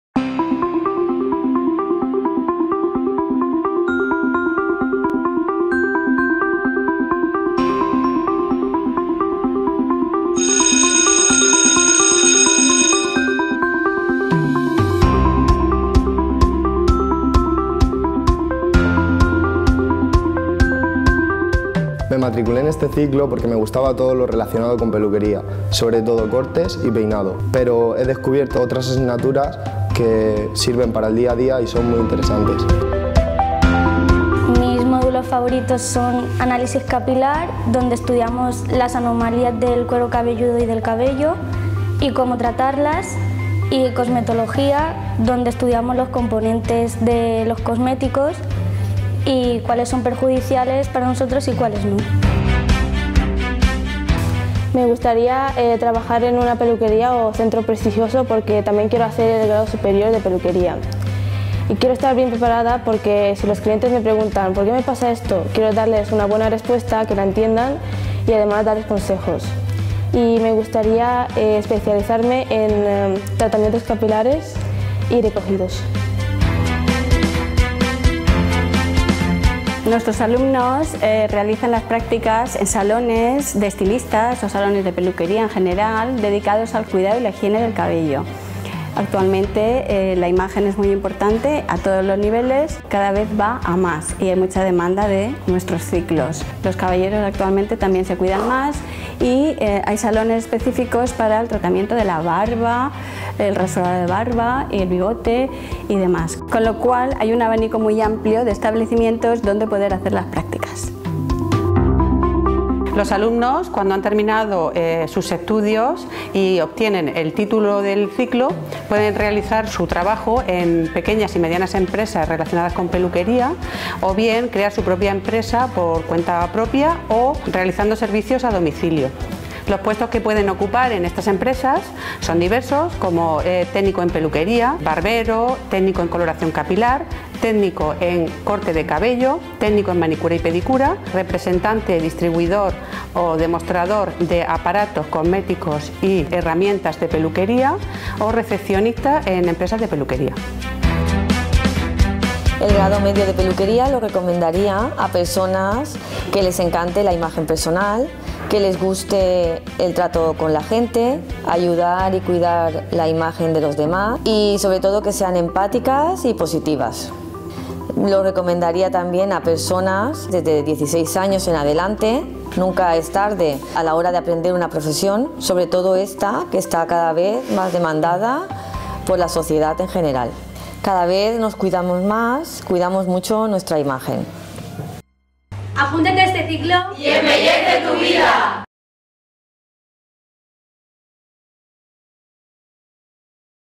Ciclo Formativo de Peluquería y Cosmética Capilar. IES. La Torreta
Vídeos realizados bajo el proyecto OSMOSIS2016/11-ORIENTAELX del Programa Ósmosis 2016-17. Profs. Responsables: Juan Francisco Reina Moreno (IES NIT DE L'ALBÀ, d'Elx) y Mª Teresa Zaragoza Fuster (UMH)